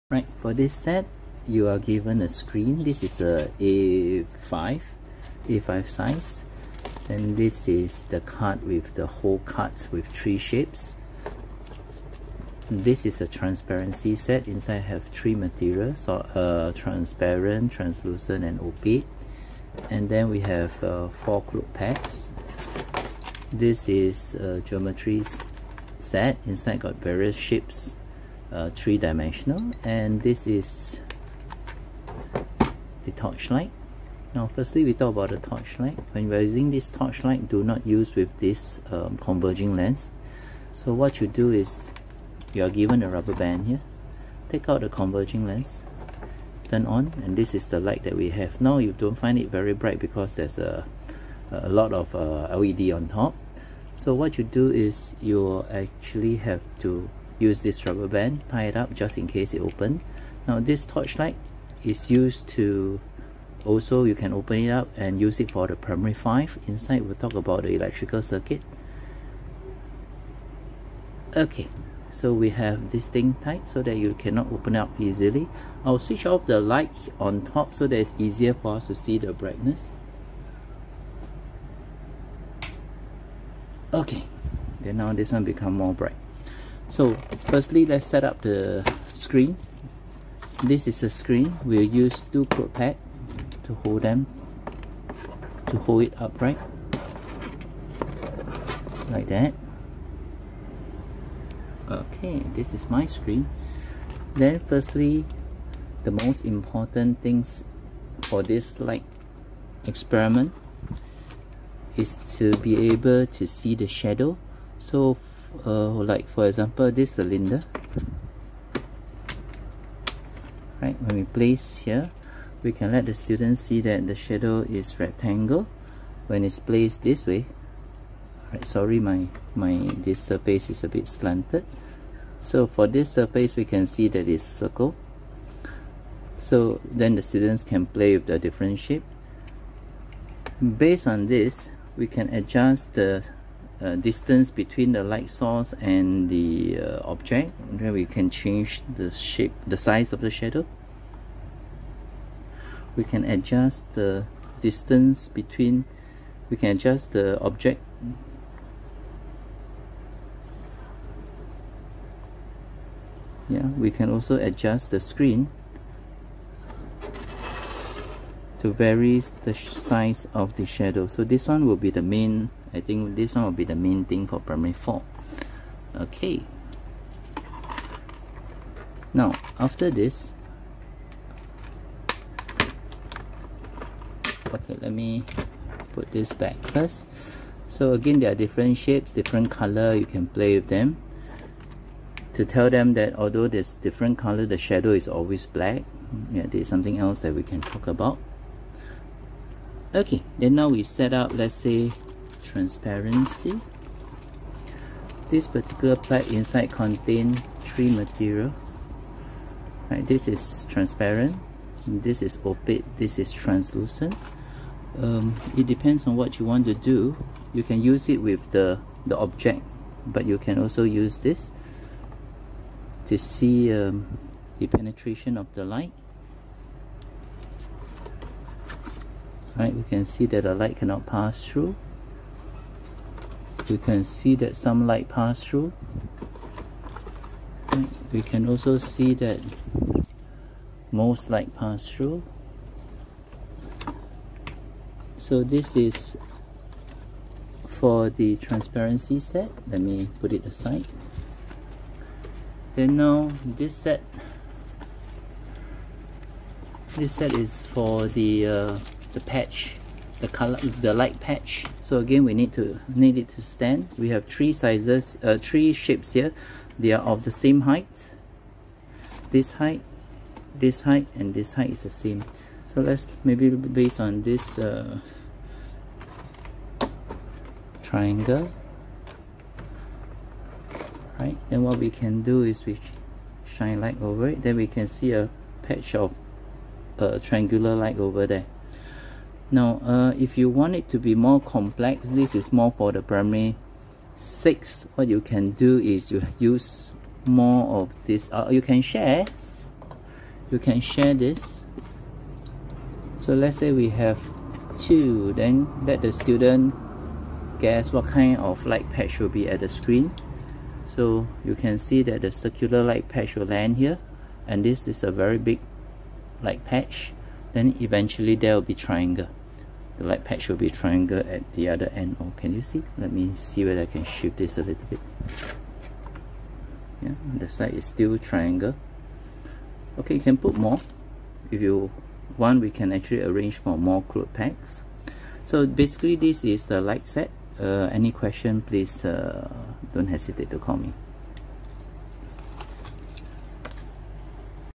light exploration set C
light exploration set C consists of the 14 pcs geometry set, LED torch light, transparency set and light patch set.